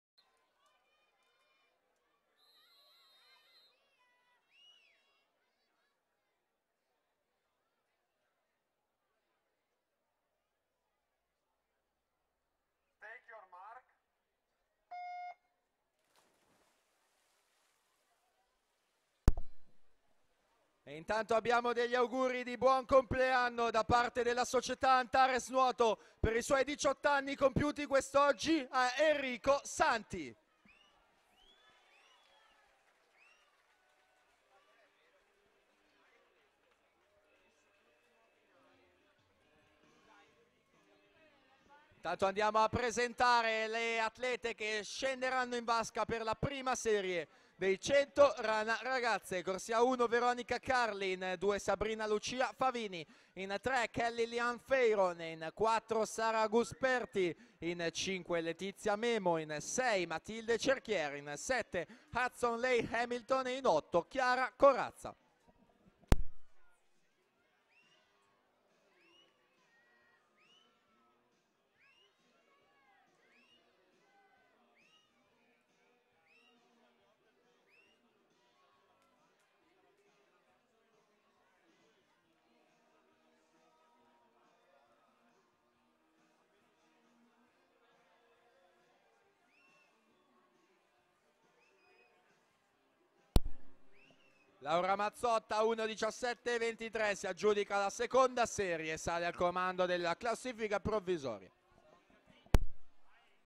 100 Rana Ragazzi Femminile (Serie 2) - Treviso Swim Cup (Swimming)
Atleti partecipanti:
in corsia 2 TUGNOLO MARTINA (2002) della soc. VIGEVANO NUOTO iscritta con 01:32.00
in corsia 3 MIKULCIC MARTINA (2002) della soc. SPORTIVAMENTE BELLUNO iscritta con 01:31.40
in corsia 4 CADEI GIORGIA (2002) della soc. OLIMPIC SWIM PRO iscritta con 01:30.00
in corsia 5 PEDERIVA SILMAVA (2002) della soc. STELLA MARINA iscritta con 01:30.00
in corsia 6 LIESSI DENISE (2002) della soc. ASD ARCA NUOTO iscritta con 01:32.00